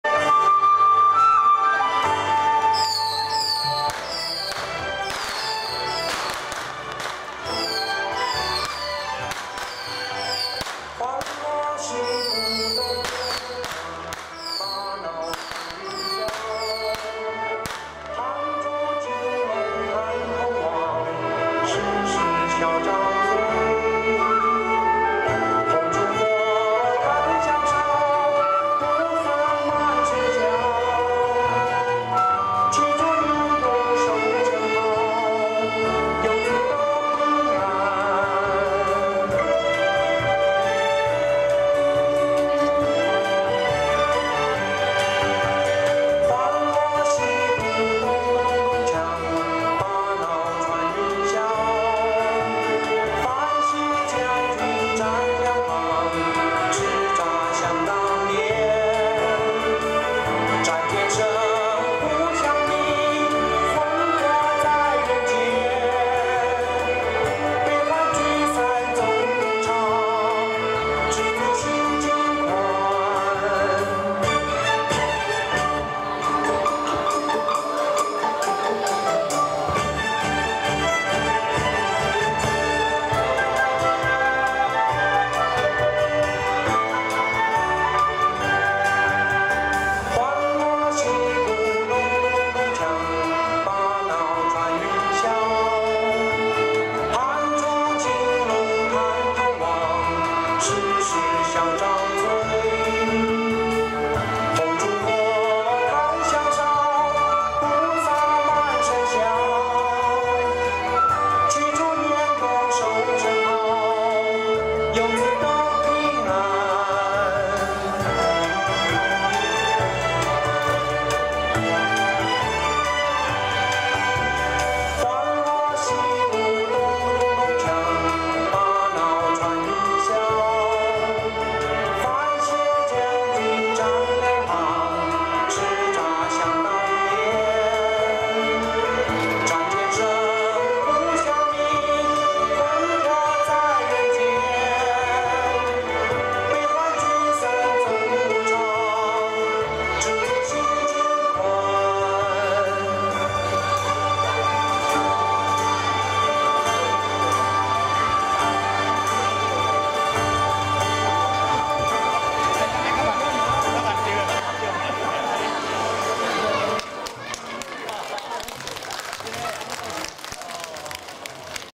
PMT 溪州復興宮蛇來運轉慶元宵：廟會 (102.2.23)
演出人員︰PMT (Play Music Team) 玩 樂 樂 團 (ㄌㄜˋ ㄩㄝˋ)。
表演主題 : 溪洲復興宮『蛇來運轉慶元宵』晚會 。
表演時間︰102年2月23日(星期六) 19:00～21:00。
表演地點︰彰化縣溪州鄉復興宮。